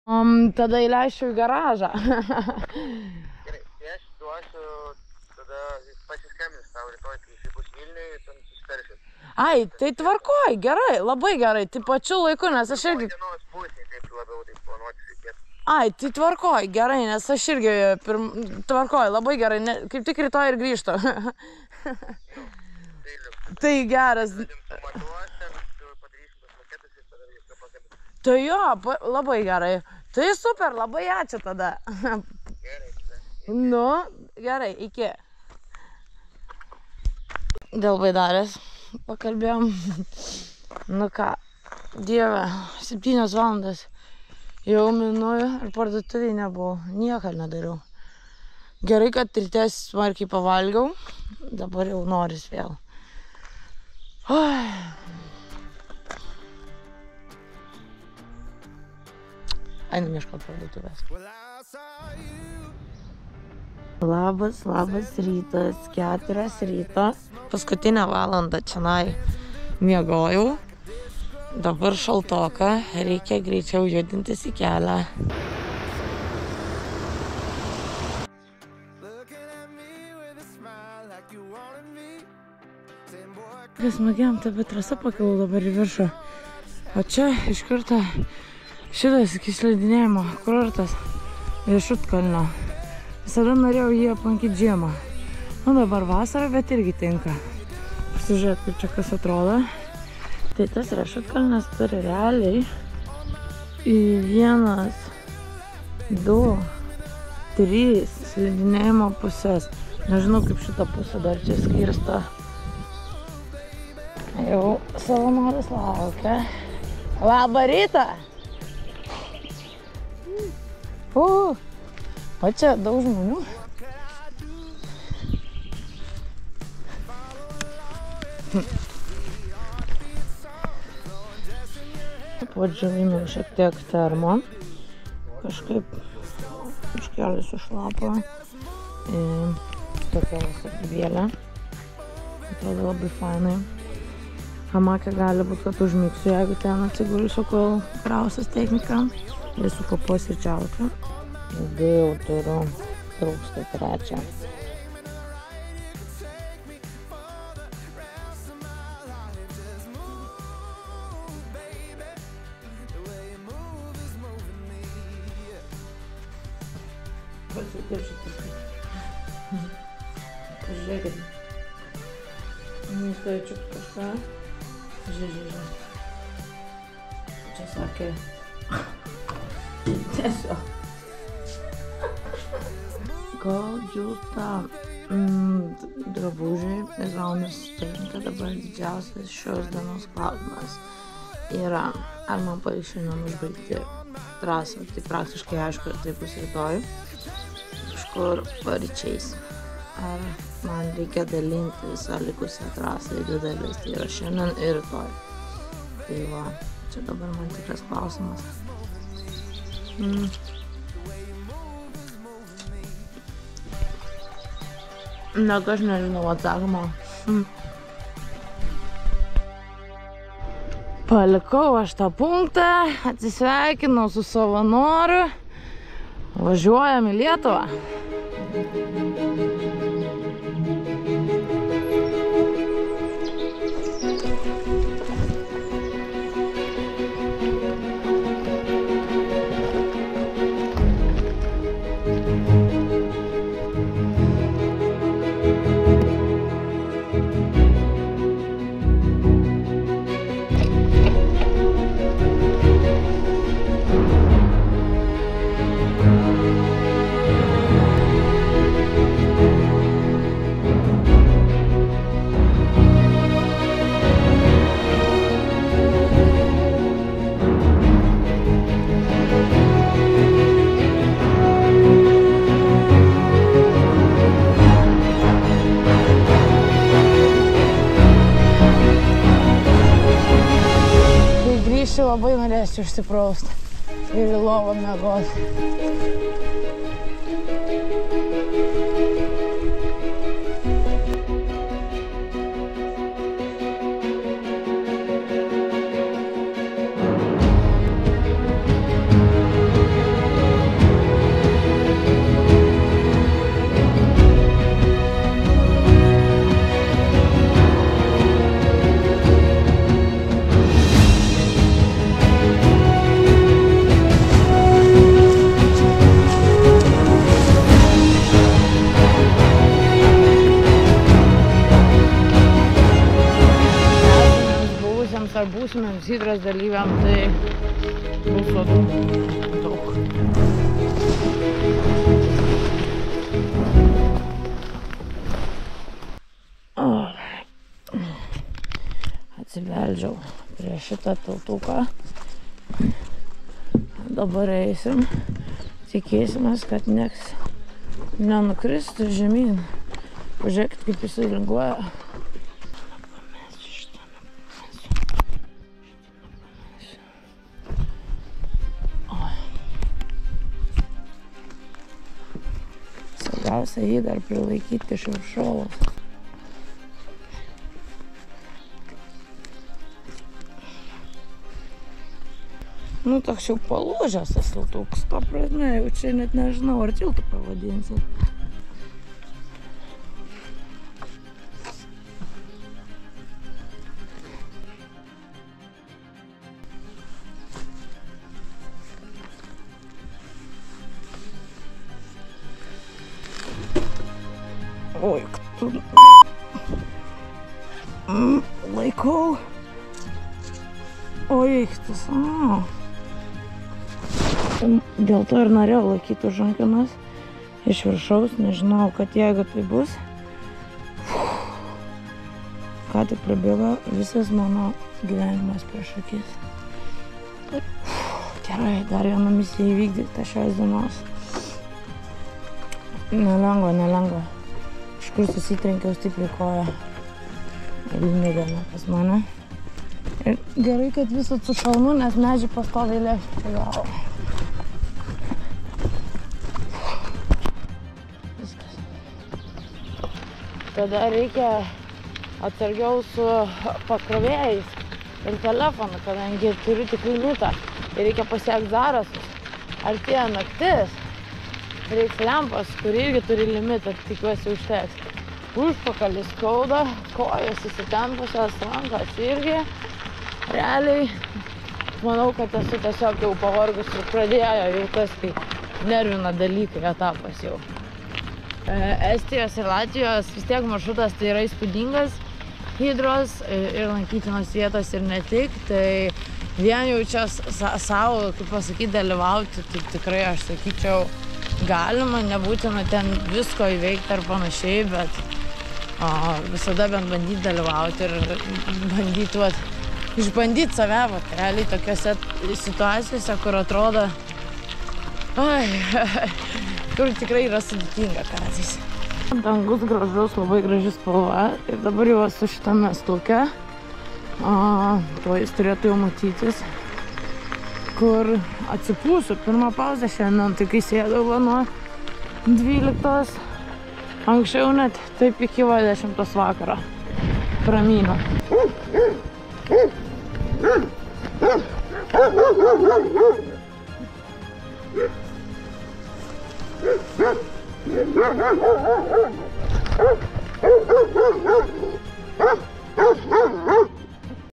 HYDRA EPIC VII 2024 | Įveikti save | 1000 km (5)
Sveiki,

Dalyvauju Hydra Epic VII.

drono video medžiaga: Tada Mikštavičius.

Swedbank:
Erika Andrilionytė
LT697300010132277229

Paysera:
Erika Andrilionytė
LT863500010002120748